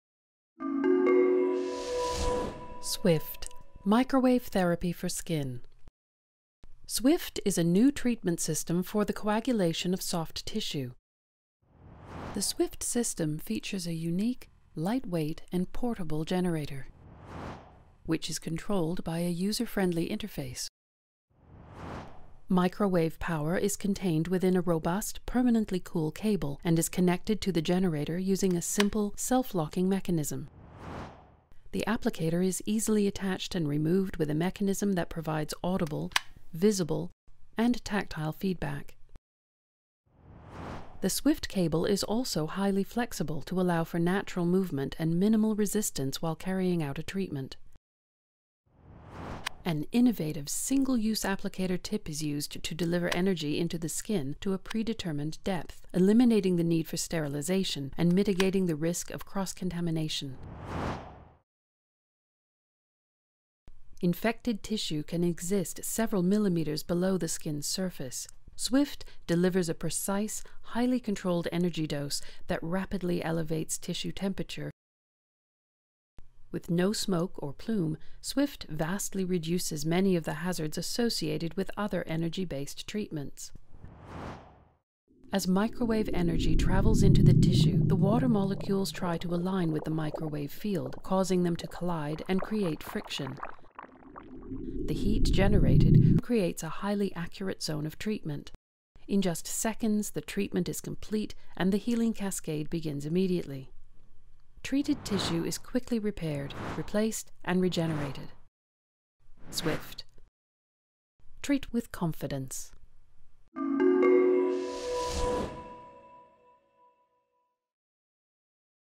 Swift Microwave Therapy System 2019
Swift is the pioneering new microwave therapy for skin. With treatment times in seconds, Swift provides Podiatrists and Dermatologists with a new, precise and easy way of treating soft tissue lesions.